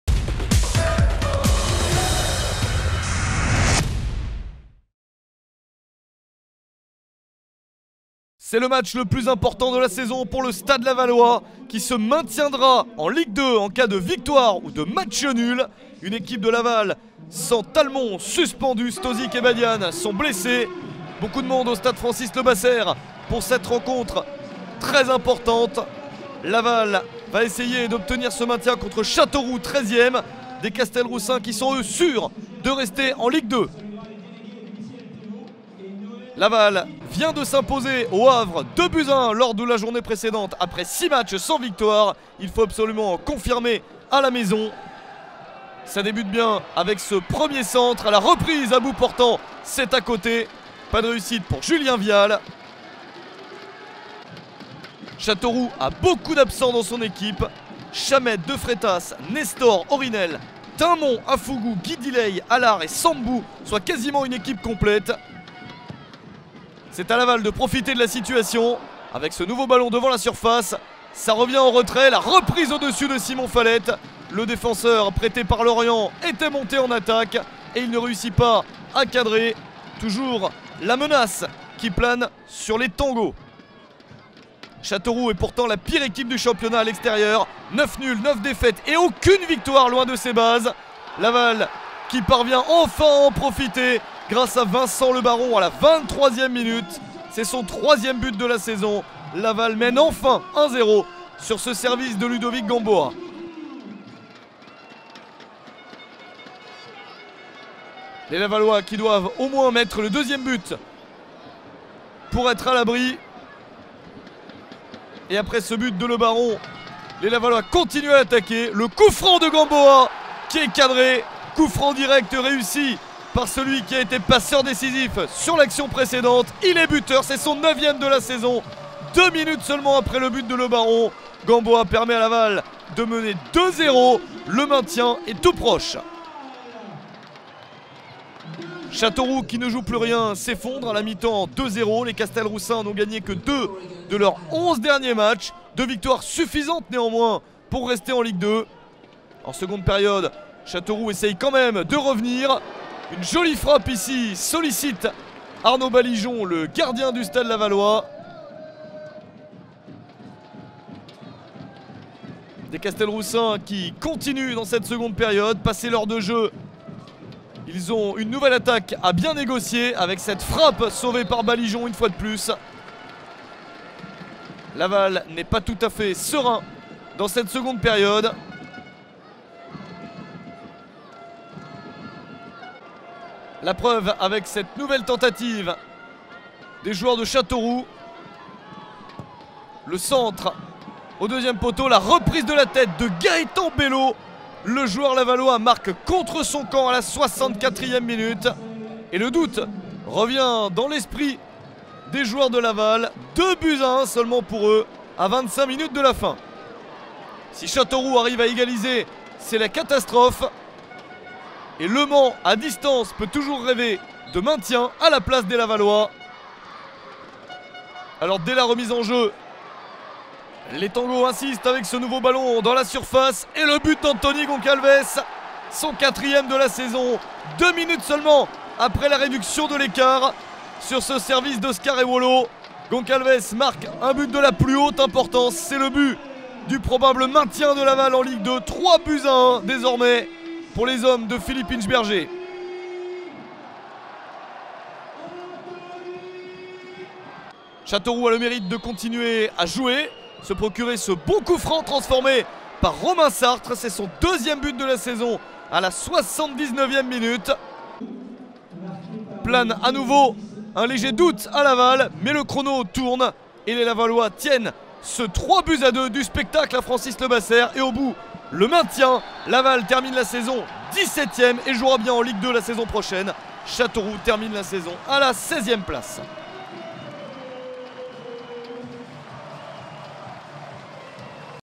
Stade Lavallois - Châteauroux (3-2) - Le résumé (LAVAL - LBC) / 2012-13
Revivez les meilleurs moments de Stade Lavallois - Châteauroux (3-2) en vidéo.

Stade Francis Le Basser - vendredi 24 mai 2013

Buteurs : Vincent LE BARON (23' - Stade Lavallois) / Ludovic GAMBOA (25' - Stade Lavallois) / Gaëtan BELAUD (64' csc - Châteauroux) / Anthony GONCALVES (66' - Stade Lavallois) / Romain SARTRE (79' - Châteauroux)

Composition Stade Lavallois : 2 - Kévin PERROT / 3 - Makan TRAORE / 4 - Oscar EWOLO (c) / 7 - Gaëtan BELAUD / 11 - Ludovic GAMBOA / 12 - Jordan ADEOTI / 15 - Yohan BETSCH / 16 - Arnaud BALIJON / 17 - Simon FALETTE / 22 - Julien VIALE / 25 - Vincent LE BARON

Composition Châteauroux : 1 - Mickael CARADEC / 3 - Fernando NEVES / 5 - Jordan FAUQUE / 6 - Romain SARTRE / 8 - Yohan HAUTCOEUR (c) / 9 - Benjamin JEANNOT / 10 - Ludovic GUERRIERO / 13 - Rémi FOURNIER / 14 - Marcel Josué ESSOMBE / 19 - Lassana DOUCOURE / 25 - Emeric DUDOUIT